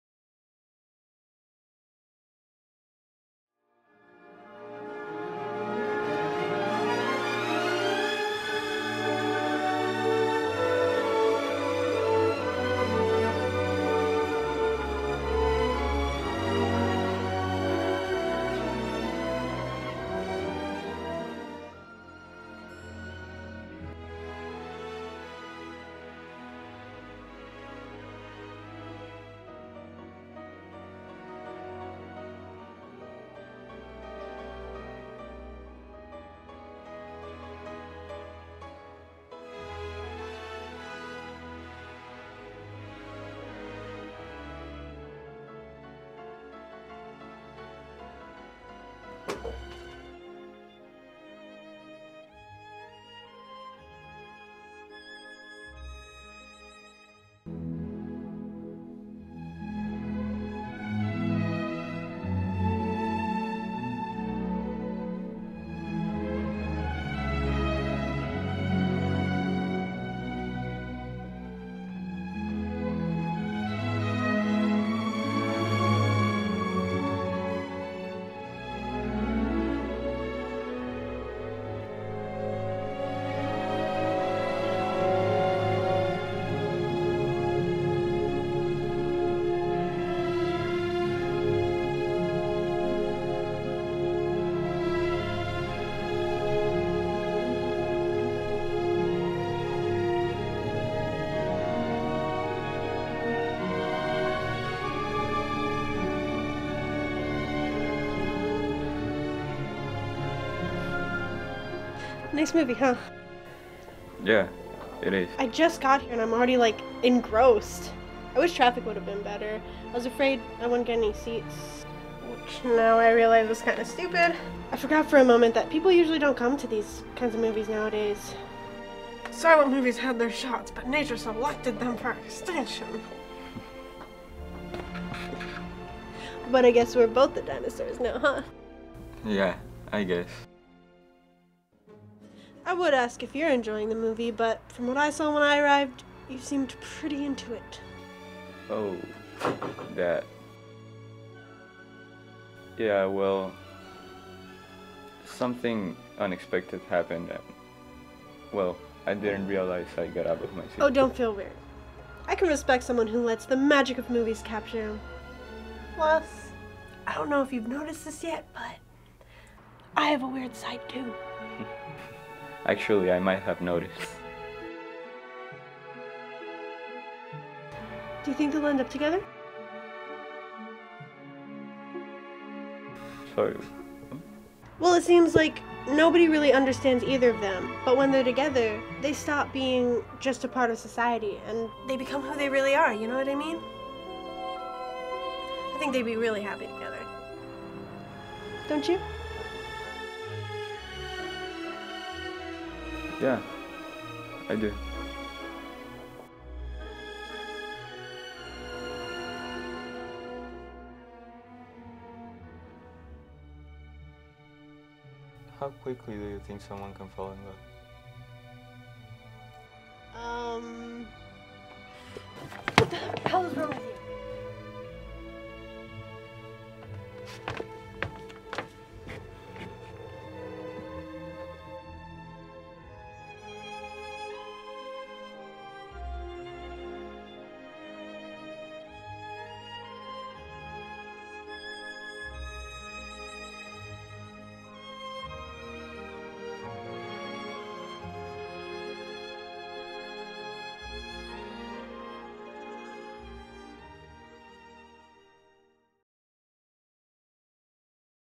"The Magic of Movies" Directing 1
Aleix Melloni's Directing 1.

A lonely film lover starts seeing the future in a theatre screen. Will it all become true?